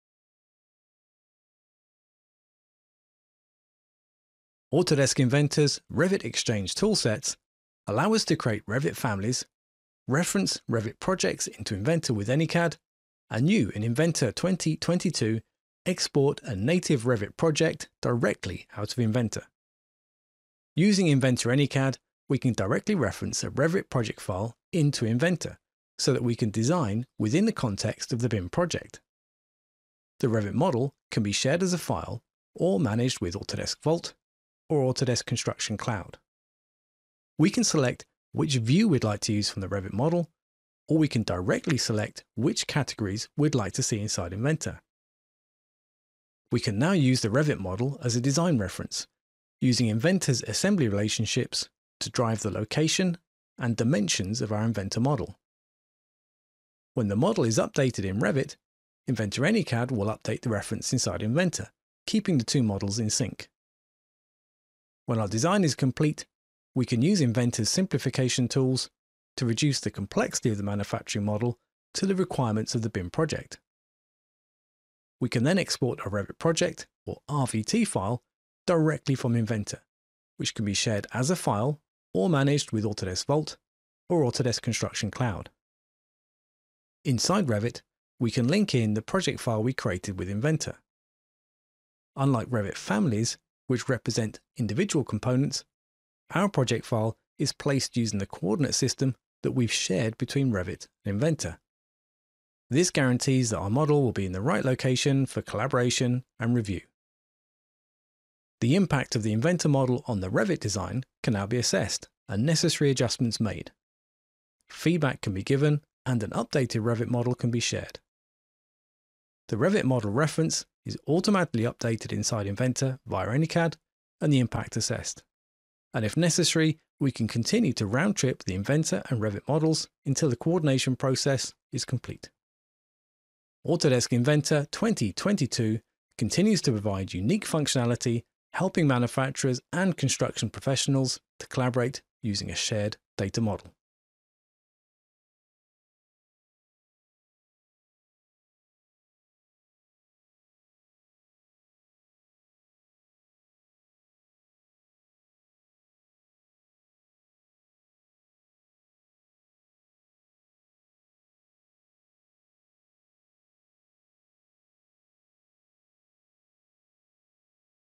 Autodesk inventor 2022: Inventor and Revit Interoperability
In addition to the ability to Reference Revit Project files into Inventor with AnyCAD, and the ability to export Revit families, Inventor 2022 can now export native Revit project files.

Export to Revit incorporates the new Simplify command to produce a Revit Project file (RVT) containing only the components and features needed for coordination.

The Revit Export and Simplify command have been combined and added to the property panel for a complete set of options in one easy-to-use interface.

Maintain associative links between Autodesk® Revit® and Inventor to improve collaboration and streamline coordination and design review on your BIM projects.